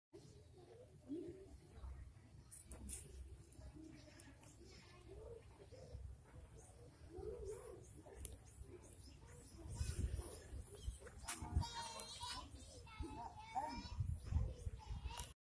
2021年6月 八木山動物公園 ヤギ・ヒツジふれあい体験でのトカラヤギ
今日2月2日から新型コロナ感染対策で、ウサギ、モルモット、ヤギ・ヒツジのふれあい体験が休止されたとのこと。
今度会うときまで、人間・動物ともに元気でありますように。